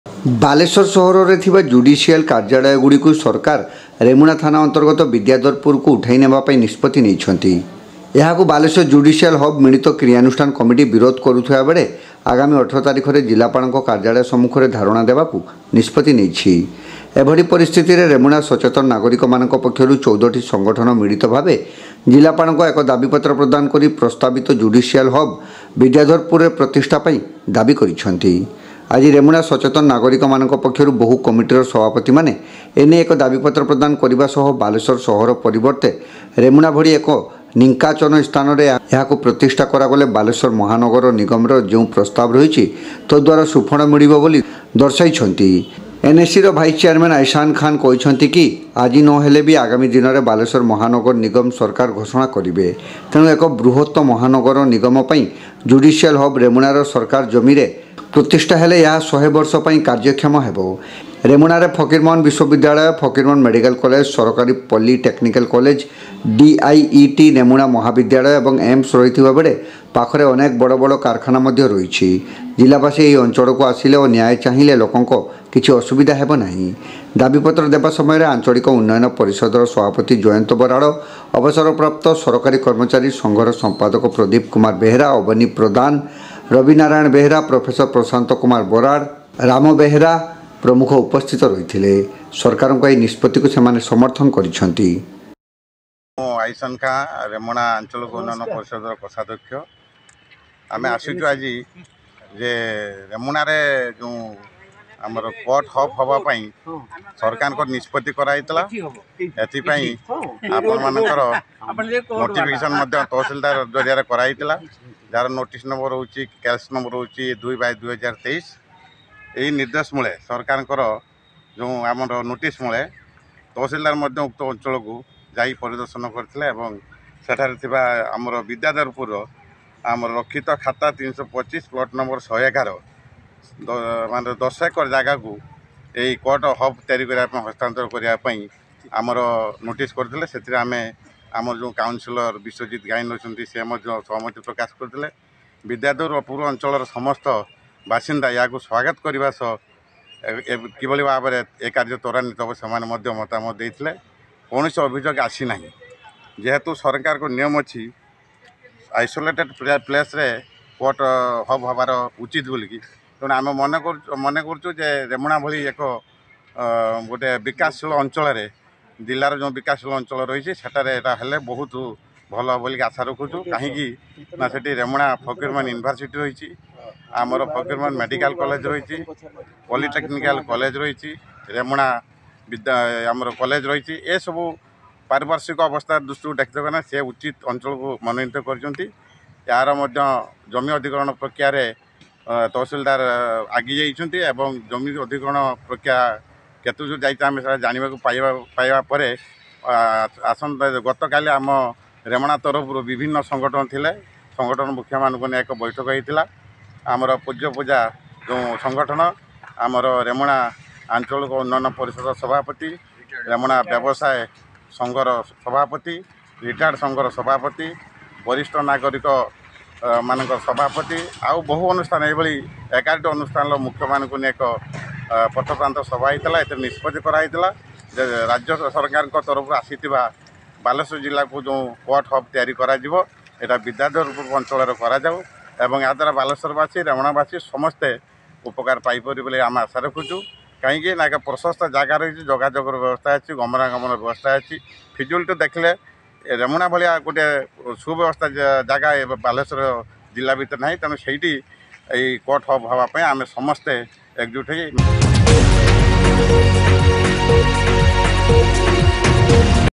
ରେମୁଣାରେ ଜୁଡ଼ିସିଆଲ ହବ୍ ପ୍ରସ୍ତାବକୁ ସମର୍ଥନ କଲେ ୧୪ଟି ସଂଗଠନ।ଜିଲ୍ଲାପାଳଙ୍କ ଜରିଆରେ ମୁଖ୍ୟମନ୍ତ୍ରୀଙ୍କୁ ଦାବିପତ୍ର
ରେମୁଣାରେ ଜୁଡ଼ିସିଆଲ ହବ୍ ପ୍ରସ୍ତାବକୁ ସମର୍ଥନ କଲେ ୧୪ଟି ସଂଗଠନ
ଜିଲ୍ଲାପାଳଙ୍କ ଜରିଆରେ ମୁଖ୍ୟମନ୍ତ୍ରୀଙ୍କୁ ଦେଲେ ଦାବିପତ୍ର
ବାଲେଶ୍ୱର/ରେମୁଣା : ବାଲେଶ୍ୱର ସହରରେ ଥିବା ଜୁଡ଼ିସିଆଲ କାର୍ଯ୍ୟାଳୟଗୁଡ଼ିକୁ ସରକାର ରେମୁଣା ଥାନା ଅନ୍ତର୍ଗତ ବିଦ୍ୟାଧରପୁରକୁ ଉଠାଇ ନେବାପାଇଁ ନଷ୍ପତି ନେଇଛନ୍ତି । ଏହାକୁ ବାଲେଶ୍ୱର ଜୁଡ଼ିସିଆଲ ହବ୍ ମିଳିତ କ୍ରିୟାନୁଷ୍ଠାନ କମିଟି ବିରୋଧ କରୁଥିବାବେଳେ ଆଗାମୀ ୧୮ ତାରିଖରେ ଜିଲ୍ଲାପାଳଙ୍କ କାର୍ଯ୍ୟାଳୟ ସମ୍ମୁଖରେ ଧାରଣା ଦେବାକୁ ନିଷ୍ପତି ନେଇଛି । ଏଭଳି ପରିସ୍ଥିତିରେ ରେମୁଣା ସଚେତନ ନାଗରିକ ମାନଙ୍କ ମକ୍ଷରୁ ୧୪ଟି ସଂଗଠନ ମିଳିତ ଭାବେ ଜିଲ୍ଲାପାଳଙ୍କ ଏକ ଦାବିପତ୍ର ପ୍ରଦାନ କରି ପ୍ରସ୍ତାବିତ ଜୁଡ଼ିସିଆଲ୍ ହବ୍ ବିଦ୍ୟାଧରପୁରରେ ପ୍ରତିଷ୍ଠା ପାଇଁ ଦାବି କରିଛନ୍ତି । ଆଜି ରେମୁଣା ସଚେତନ ନାଗରିକମାନଙ୍କ ପକ୍ଷରୁ ବହୁ କମିଟିର ସଭାପତିମାନେ ଏନେଇ ଏକ ଦାବିପତ୍ର ପ୍ରଦାନ କରିବା ସହ ବାଲେଶ୍ୱର ସହର ପରିବର୍ତେ ରେମୁଣା ଭଳି ଏକ ନିକାଂଚନ ସ୍ଥାନରେ ଏହାକୁ ପ୍ରତିଷ୍ଠା କରାଗଲେ ବାଲେଶ୍ୱର ମହାନଗର ନିଗମର ଯେଉଁ ପ୍ରସ୍ତାବ ରହିଛି ତଦ୍ୱାରା ସୁଫଳ ମିଳିବ ବୋଲି ଦର୍ଶାଇଛନ୍ତି । ଏନଏସିର ଭାଇସ୍ ଚେୟାରମ୍ୟାନ ଏହେସାନ ଖାନ୍ କହିଛନ୍ତି କି, ଆଜି ନହେଲେ ବି ଆଗାମୀ ଦିନରେ ବାଲେଶ୍ୱର ମହାନଗର ନିଗମ ସରକାର ଘୋଷଣା କରିବେ । ତେଣୁ ଏକ ବୃହତ ମହାନଗର ପାଇଁ ଜୁଡ଼ିସିଆଲ ହବ୍ ରେମୁଣାର ସରକାର ଜମିରେ ପ୍ରତିଷ୍ଠା ହେଲେ ଏହା ଶହେବର୍ଷ ପାଇଁ କାର୍ଯ୍ୟକ୍ଷମ ହେବ । ଏନଏସିର ୭ ନମ୍ବର ୱାର୍ଡ଼ର ଖାତା ନଂ ୩୨୫(ରଖିତ), ପ୍ଲଟ୍ ନଂ ୧୧୧ରେ ଥିବା ୧୦ ଡିସିମିଲ୍ ଜମି ଏଥିପାଇଁ ହସ୍ତାନ୍ତର ଲାଗି ଅଗଷ୍ଟ ୨୧ରେ ବିଜ୍ଞପ୍ତି ପ୍ରକାଶ ପାଇଥିଲା । ଉକ୍ତ ମୌଜାର ଗ୍ରାମବାସୀ ମଧ୍ୟ ୧୫ ଦିନ ମଧ୍ୟରେ କୌଣସି ଆପତି ଅଭିଯୋଗ କରିନଥିଲେ । ରେମୁଣାରେ ଫକୀରମୋହନ ବିଶ୍ୱବିଦ୍ୟାଳୟ, ଫକୀରମୋହନ ମେଡ଼ିକାଲ କଲେଜ୍‌, ସରକାରୀ ପଲ୍ଲି ଟେକ୍ନିକାଲ କଲେଜ୍‌, ଡିଆଇଇଟି, ରେମୁଣା ମହାବିଦ୍ୟାଳୟ ଏବଂ ଏମ୍ସ ରହିଥିବା ବେଳେ ପାଖରେ ଅନେକ ବଡ଼ବଡ଼ କାରଖାନା ମଧ୍ୟ ରହିଛି । ଜିଲ୍ଲାବାସୀ ଏହି ଅଂଚଳକୁ ଆସିଲେ ଓ ନ୍ୟାୟ ଚାହିଁଲେ ଲୋକଙ୍କ କିଛି ଅସୁବିଧା ହେବନାହିଁ । ଦାବିପତ୍ର ଦେବା ସମୟରେ ଆଂଚଳିକ ଉନ୍ନୟନ ପରିଷଦ ସଭାପତି ଜୟନ୍ତ ବରାଳ, ଅବସରପ୍ରାପ୍ତ ସରକାରୀ କର୍ମଚାରୀ ସଂଘର ସମ୍ପାଦକ ପ୍ରଦୀପ କୁମାର ବେହେରା, ଅବନୀ ପ୍ରଦାନ, ରବି ନାରାୟଣ ବେହେରା, ପ୍ରଫେସର ପ୍ରଶାନ୍ତ କୁମାର ବରାଳ ପ୍ରମୁଖ ଉପସ୍ଥିତ ରହିଥିଲେ । ସରକାରଙ୍କ ଏହି ନିଷ୍ପତିକୁ ସେମାନେ ସମର୍ଥନ କରିଛନ୍ତି ।